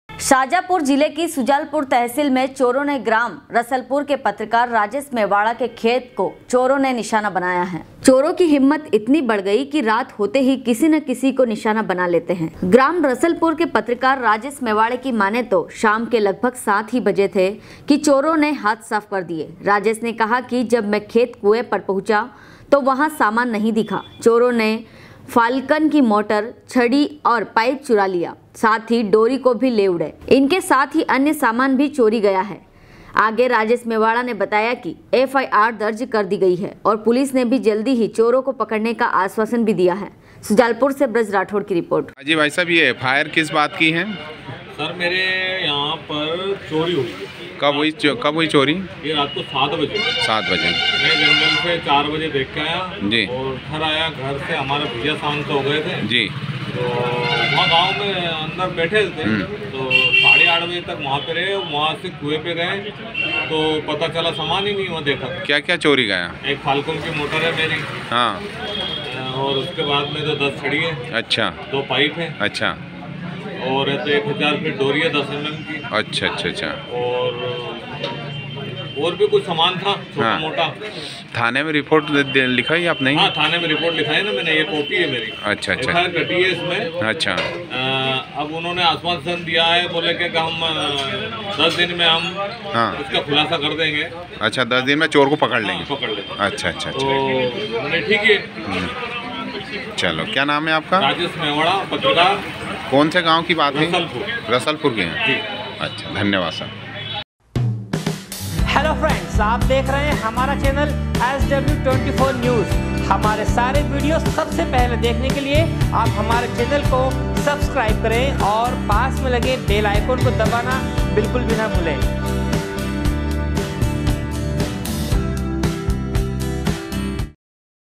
शाजापुर जिले की शुजालपुर तहसील में चोरो ने ग्राम रसलपुर के पत्रकार राजेश मेवाड़ा के खेत को चोरों ने
शाजापुर जिले की शुजालपुर तहसील में चोरो ने ग्राम रसलपुर के पत्रकार राजेश मेवाड़ा के खेत को चोरों ने निशाना बनाया है
शुजालपुर से ब्रज राठौर की रिपोर्ट।